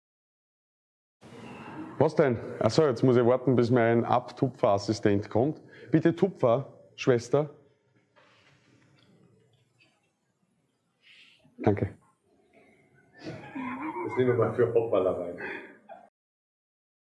Hoppala from Set - Tutorial Squib Load
Kleines "Hoppala" vom Dreh des Squib Load Tutorials. Darf ich vorstellen, mein Assisten Paddy.